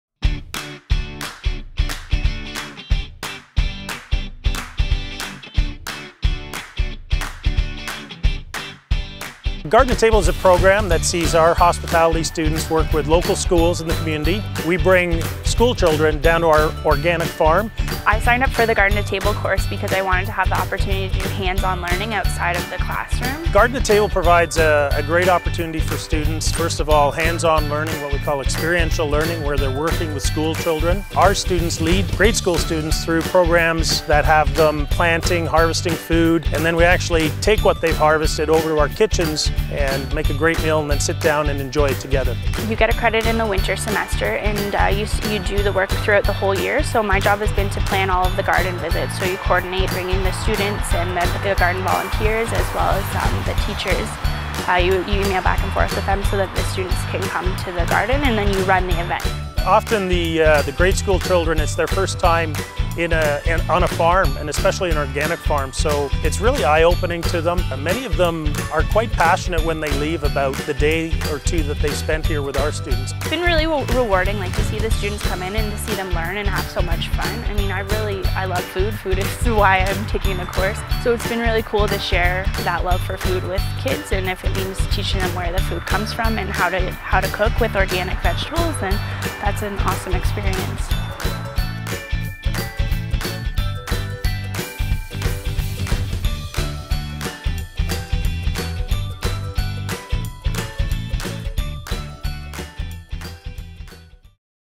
BComm Experience: Garden2Table
Food education and research is alive and thriving at the University of Guelph. The Garden2Table program gives students the opportunity to educate youth about where their food comes from. The video is filmed at the Guelph Centre for Urban Organic Farming.

Transcript

VIDEO: The video starts with music and footage of various University of Guelph landmarks including the University of Guelph sign, science complex, cannon and Winegard Walk. Text appears “B.Comm Experience Garden 2 Table.”

BRUCE: Garden2Table is a program that sees our hospitality students work with local schools in the community we bring school children down to our organic farm.

BONNIE: I signed up for the Garden2Table course because I wanted to have the opportunity to do hands-on learning outside of the classroom.

BRUCE: Garden2Table provides a great opportunity for students first of all hands on learning what we call experiential learning where they're working with school children. Our students lead grade school students through programs that have them planting harvesting food and then we actually take what they've harvested over to our kitchens and make a great meal and sit down and enjoy it together.

BONNIE: You get a credit in the winter semester and you do the work throughout the whole year so my job has been to plan all of the garden visits so you coordinate bringing the students in and the garden volunteers as well as the teachers you email back and forth with them so that the students can come to the garden and then you run the event.

BRUCE: Often the grade school children it's their first time on a farm and especially an organic farm so it's really eye-opening to them and many of them are quite passionate when they leave about the day or two that they spent here with our students.

BONNIE: It's been really rewarding to see the students come in and see them learn and have so much fun I mean I really I love food; food is why I'm taking the course so it's been really cool to share that love for food with kids and if it means teaching them where the food comes from and how to cook with organic vegetables then that's an awesome experience.

VIDEO: Footage of the University of Guelph campus and of business students featured in all of the BComm experience videos. Text appears: “Be a part of our community. Guelph’s Bachelor of Commerce program.”